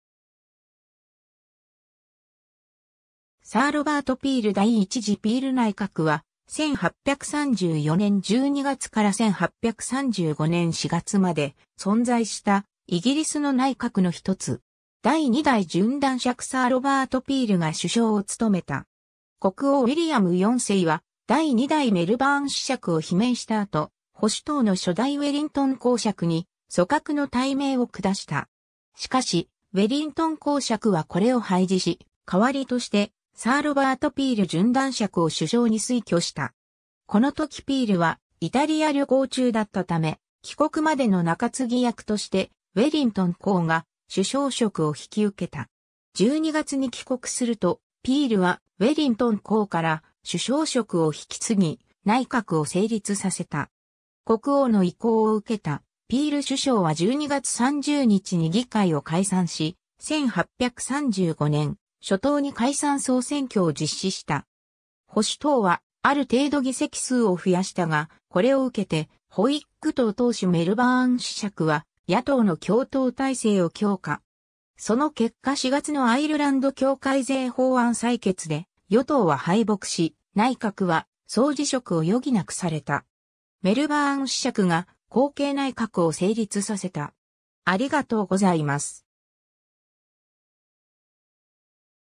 第1次ピール内閣
ロバート・ピール
アーサー・ウェルズリー
サー・ロバート・ピール 第1次ピール内閣（だいいちじピールないかく、英語: First Peel ministry）は、1834年12月から1835年4月まで存在したイギリスの内閣の一つ。
第2代準男爵サー・ロバート・ピールが首相を務めた。
 国王ウィリアム4世は第2代メルバーン子爵を罷免した後、保守党の初代ウェリントン公爵に組閣の大命を降した。
しかしウェリントン公爵はこれを拝辞し、代わりとしてサー・ロバート・ピール準男爵を首相に推挙した。
この時ピールはイタリア旅行中だったため、帰国までの中継ぎ役としてウェリントン公が首相職を引き受けた（ウェリントン公爵暫定内閣、1834年11月-12月）。
 12月に帰国するとピールはウェリントン公から首相職を引き継ぎ、内閣を成立させた。
国王の意向を受けたピール首相は12月30日に議会を解散し、1835年初頭に解散総選挙を実施した。
保守党はある程度議席数を増やしたが、これを受けてホイッグ党党首メルバーン子爵は野党の共闘態勢を強化。
その結果4月のアイルランド教会税法案採決で与党は敗北し、内閣は総辞職を余儀なくされた。
メルバーン子爵が後継内閣を成立させた。